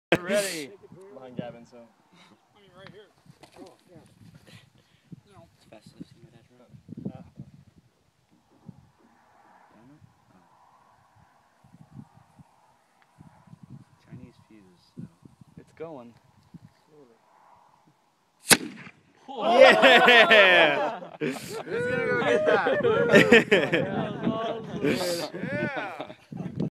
Mini Cannon
Shooting guns and cannons on new years. or i think it was new years.